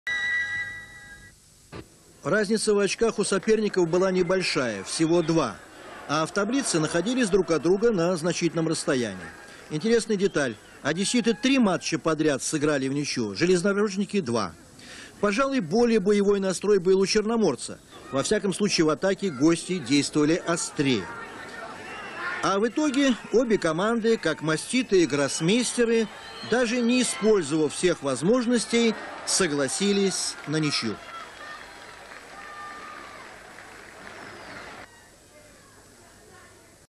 Локомотив 0-0 Черноморец. Чемпионат СССР 1991
Локомотив (Москва) 0-0 Черноморец (Одесса)
Чемпионат СССР 1991. 14-й тур.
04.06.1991
Москва, стадион "Локомотив"